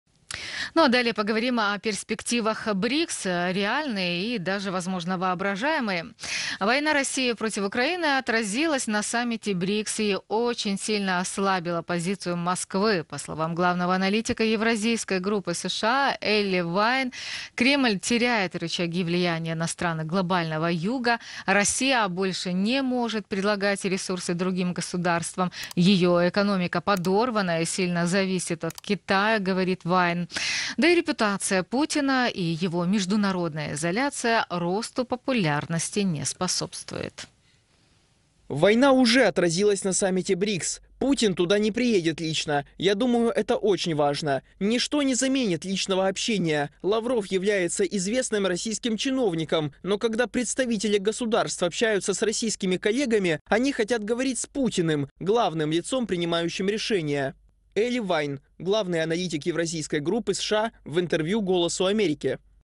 Экономика РФ подорвана: как война россии против Украины отразилась на саммите Brics
Война рф против Украины отразилась на саммите Brics и очень сильно слабила позицию Москвы. Кремль теряет рычаги влияния на страны Глобального Юга. Россия также не может предлагать ресурсы другим государствам. Подробнее смотрите в нашем видео.